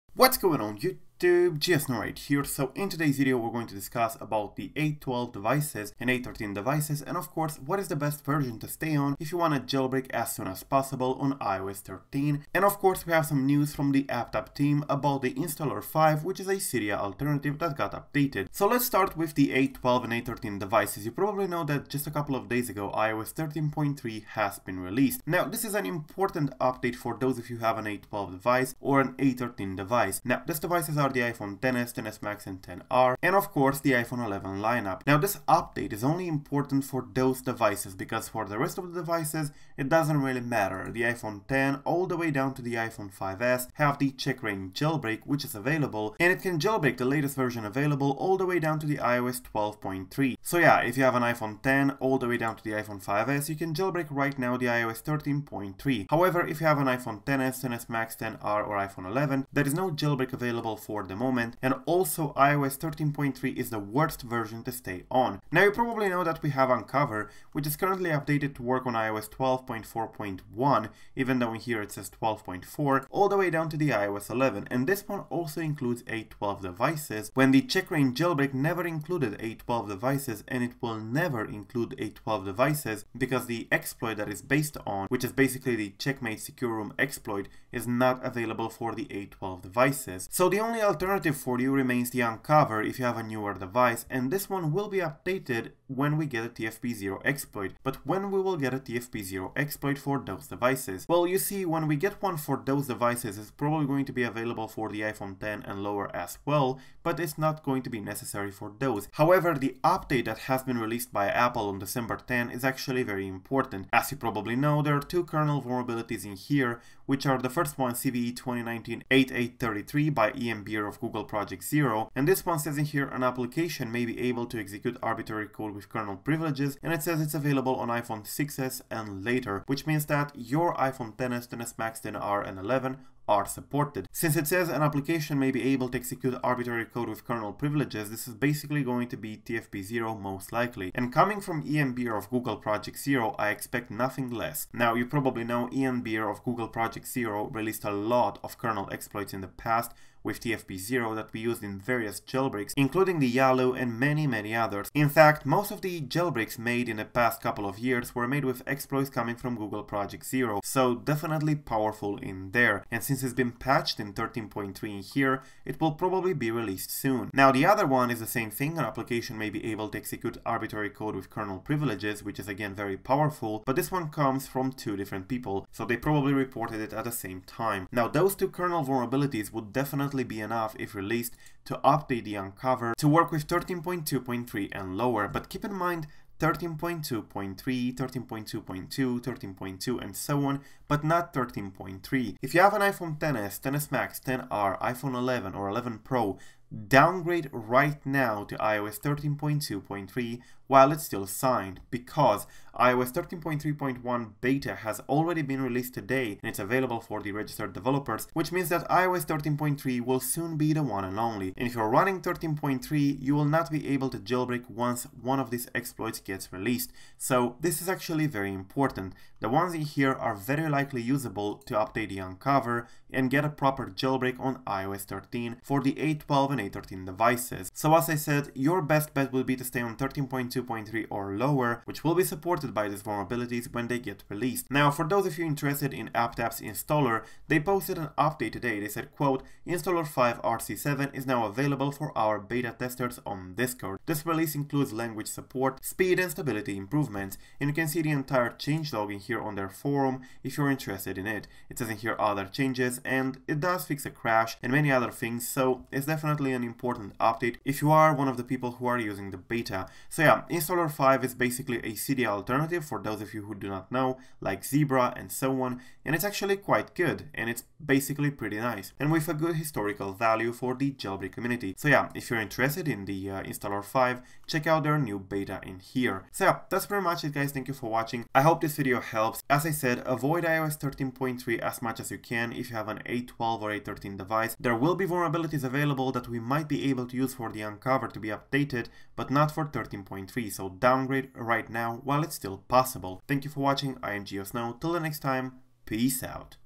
iOS 13.3 / 13.2.3 / 13.0 A12 / A13 JAILBREAK IMPORTANT Update & Installer 5 New Beta
▶ Forgot iPhone passcode? iMyFone LockWiper helps you remove Apple ID, lock screen or Screen Time without a password.

In today's video, we're discussing important recommendations regarding the iOS 13.3, iOS 13.2.3, iOS 13.2, iOS 13.0 and so on Jailbreak for A12 and A13 devices (iPhone XS, XS Max, XR, and iPhone 11 / 11 Pro). We're discussing the best version to stay on, what version you should downgrade to, why are those recommended and when we should expect new kernel exploits to update Unc0ver. We're also discussing a new update to Installer 5, a Cydia Alternative by AppTapp team.

It's always very important to stay on the proper iOS version if you wanna jailbreak your device as soon as possible. See, iPhone X and lower users don't have to worry about the right iOS version or new exploits because CheckRa1n Jailbreak pretty much got them covered for life. For the new and relevant devices (A12 and A13) CheckRa1n is useless because it's not compatible and it will never be due to the nature of the exploit used, so for these devices staying on proper iOS versions is key. It can make the difference between jailbreaking soon or jailbreaking in 6 months.